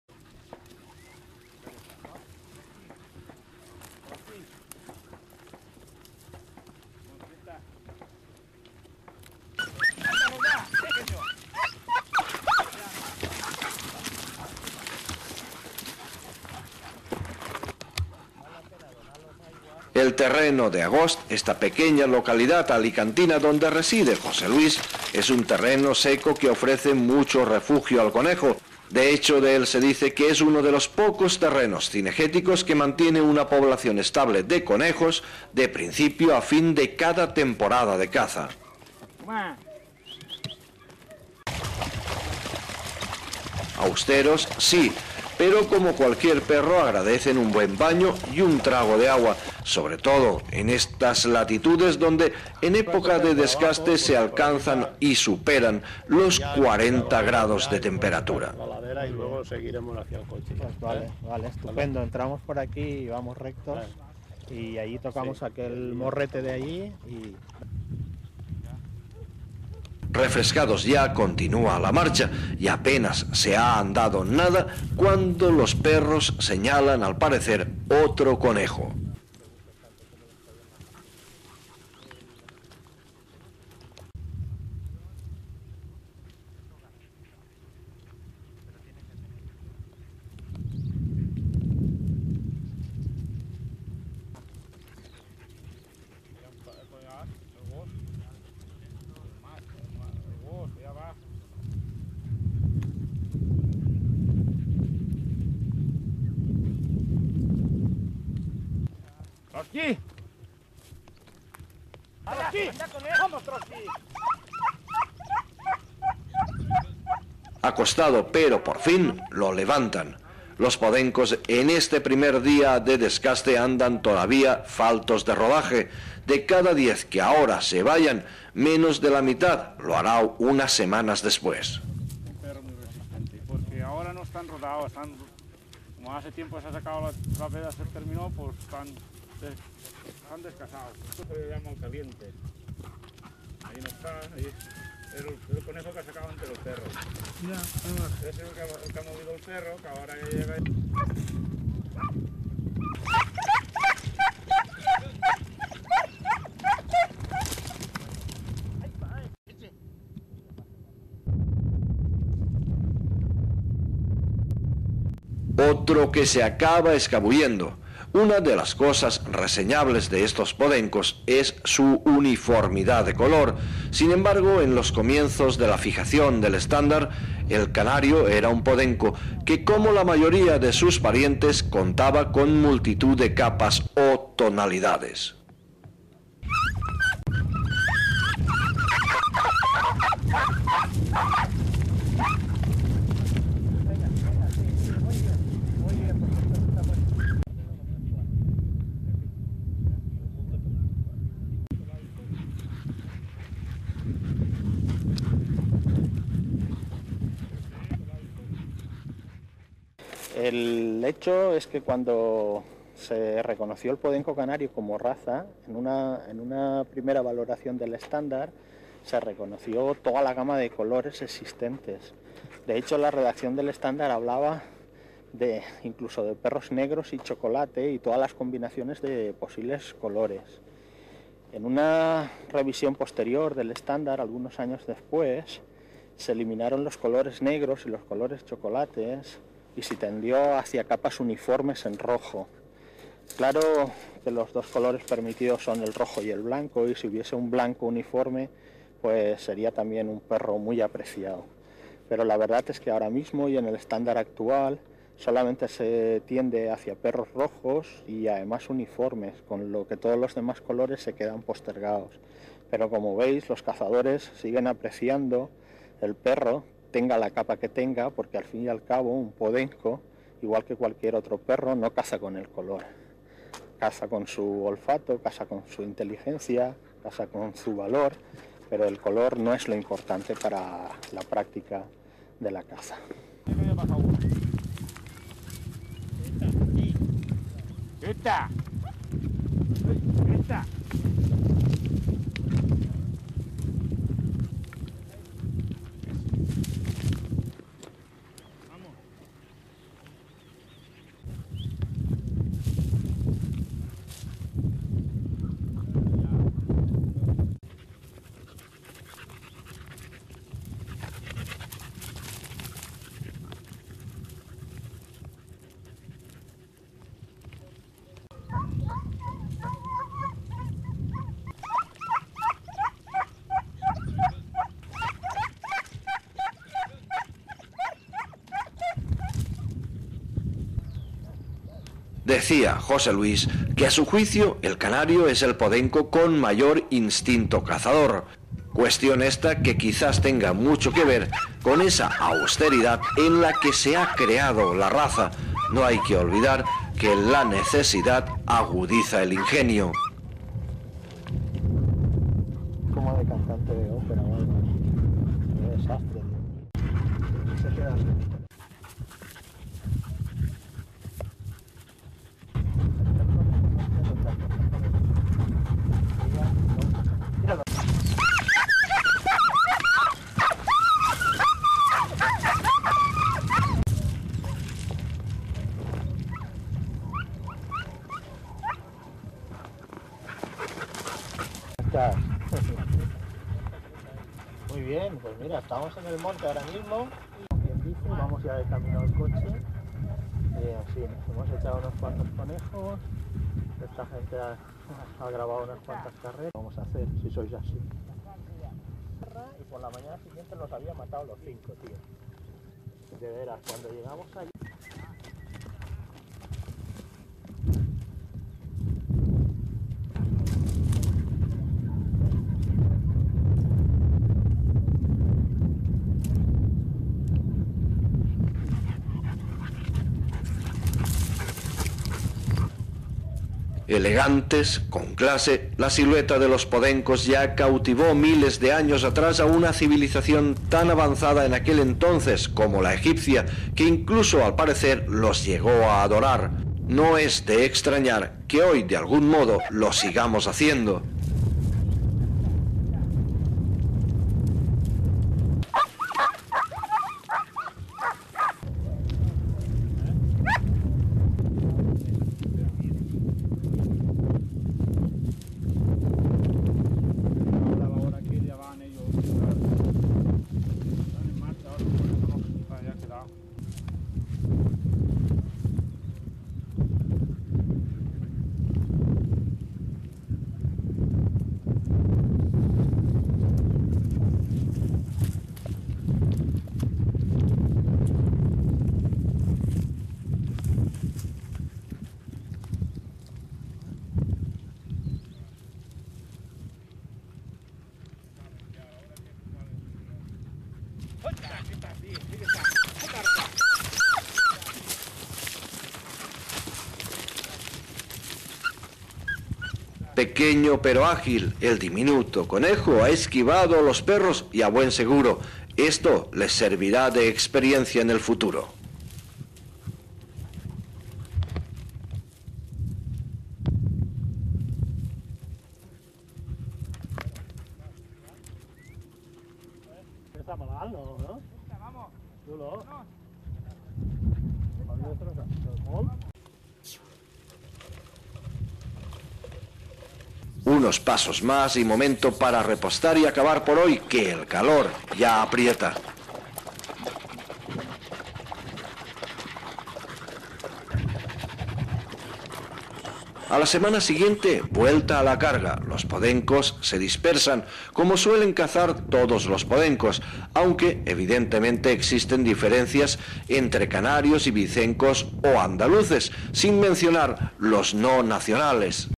Podenco Canario - Canera de Mouclem 2
Cazando con Podencos Canarios. Criador José Luis Vicedo Castellô, Canera de Mouclem. Hunting with Podencos Canarios. Breeder José Luis Vicedo Castello. Kennel Canera de Mouclem.